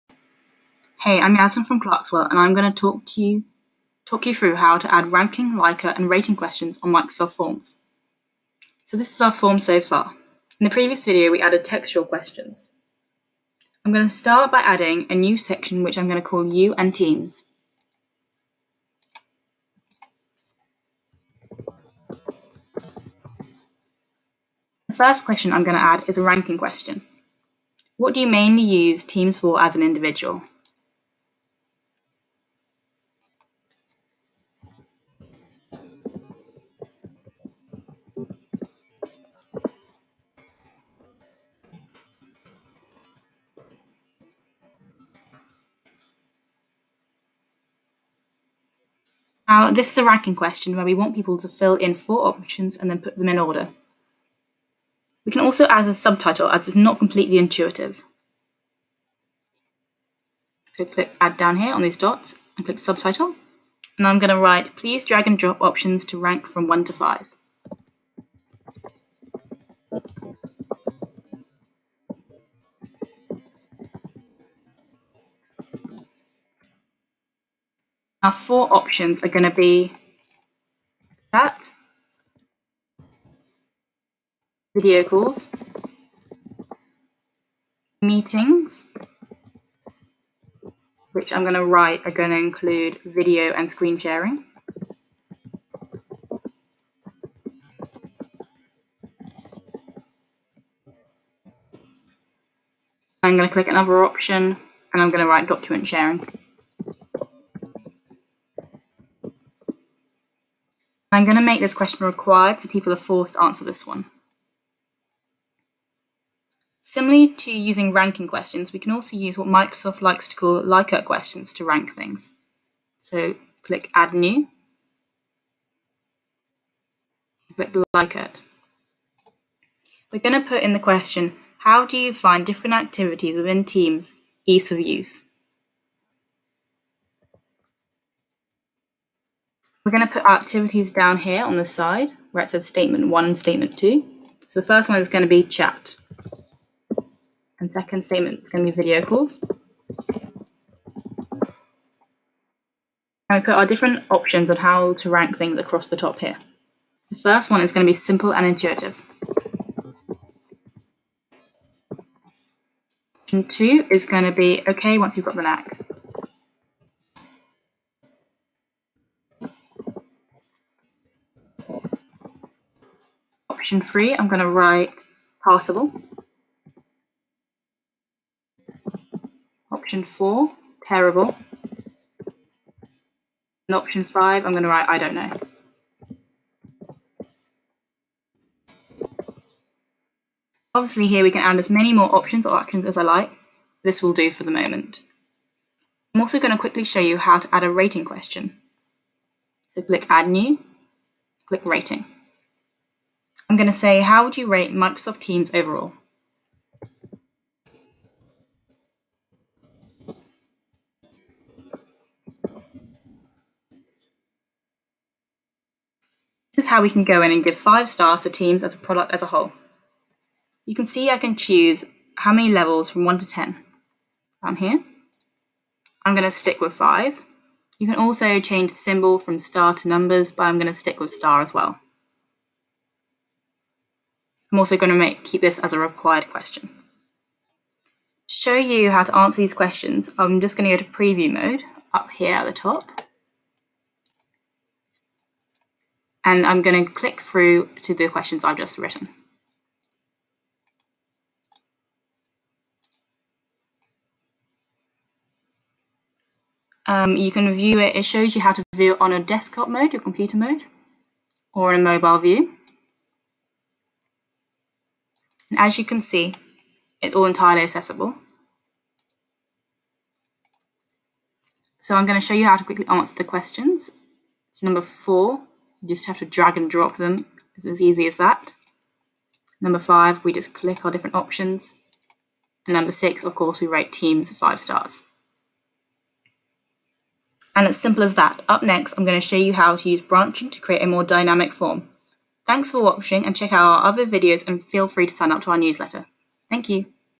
How to add Ranking, Likert, and Rating questions in Microsoft Forms
The third video in our Microsoft Forms mini-series where we run you through how to add ranking, likert and rating questions to your Form.